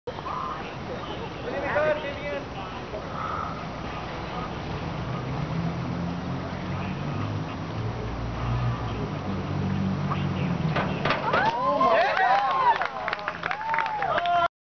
inward dive
definitely not the best form but was a success for my first time going off a 10 ft diving board :)